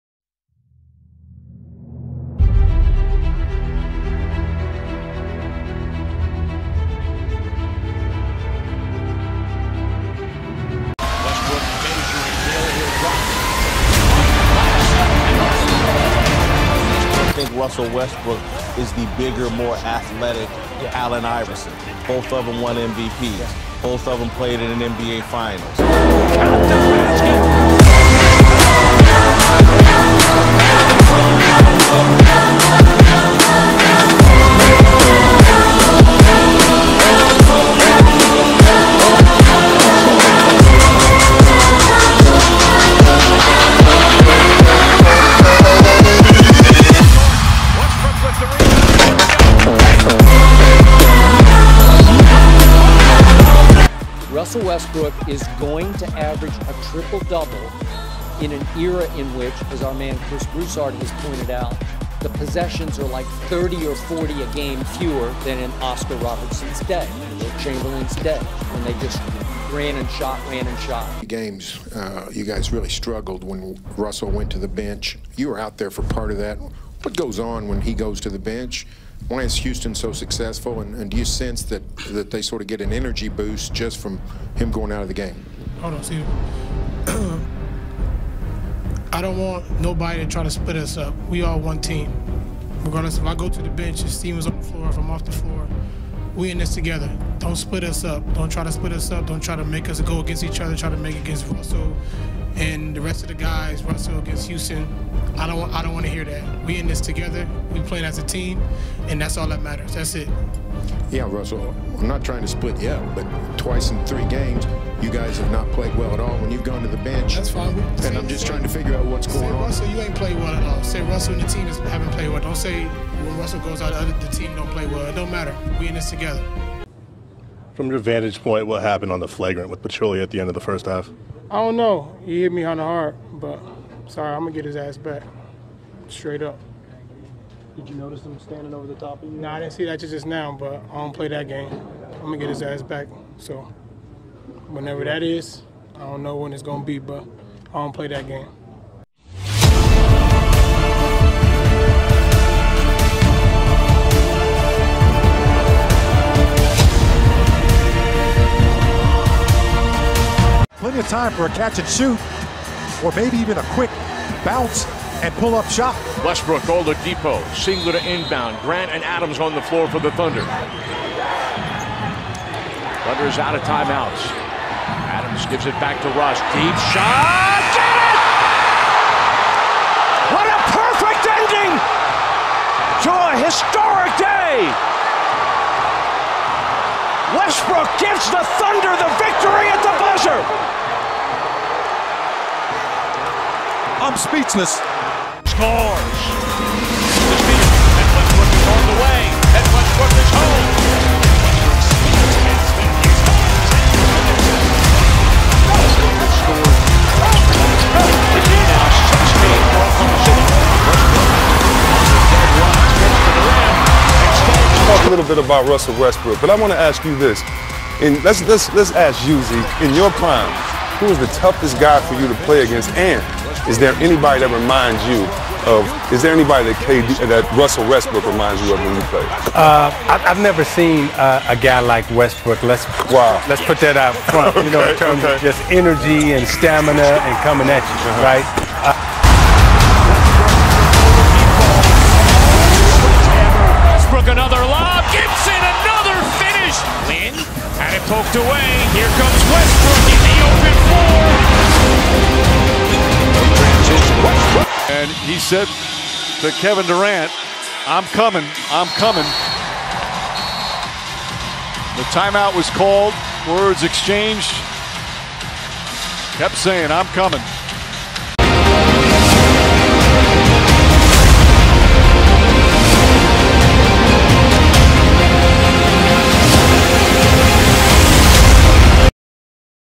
Westbrook, Short Movie, MVP Season 2016/2017
This video is about Russell Westbrook. He won MVP award in season 2016/2017 and he averaged triple-double. This video contains the highlights and some quotes about Russell Westbrook and one press conference. I hope you will enjoy in this video. See you soon!  :)